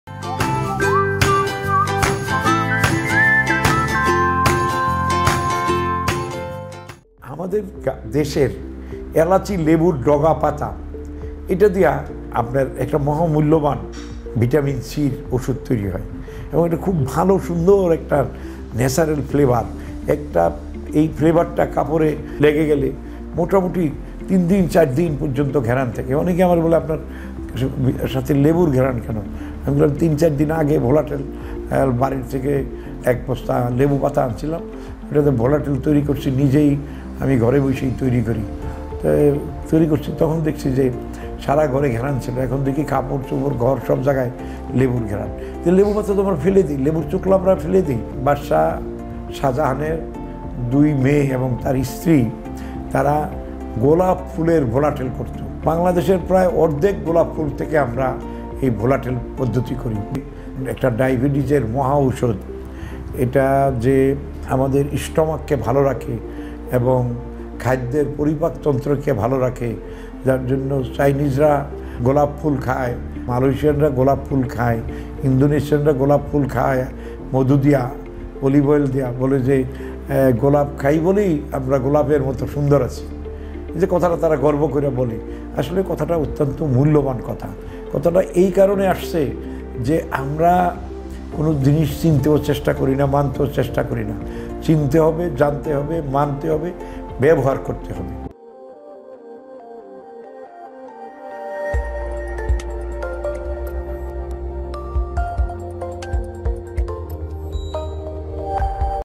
বাদশা শাহজাহান ও তার মেয়ে গোলাপ ফুল দিয়ে ভোলাটেল করতো l Rose Uses l Dr.AlamgirMati Modern Herbal
বাদশা শাহজাহান ও তার দুই মেয়ে

গোলাপফুল দিয়ে যা করতো l ডা.আলমগীর মতি

মোবাইলঃ ০১৯১-১৩৮-৬৬১৭
বিনা ভিজিটে চিকিৎসা নিন।
ডা.মুকুল-০১৭১-২৫৬-১৬৭৯
ডা.মুকুল-০১৭১২৫৬১৬৭৯
ডা.হানিফ-০১৭১৫২২৯৩৪৭
ডা.আসমা-০১৬৮১৩৫৫০৮৬
ডা.জুয়েল-০১৭১৬৫৬৪১০১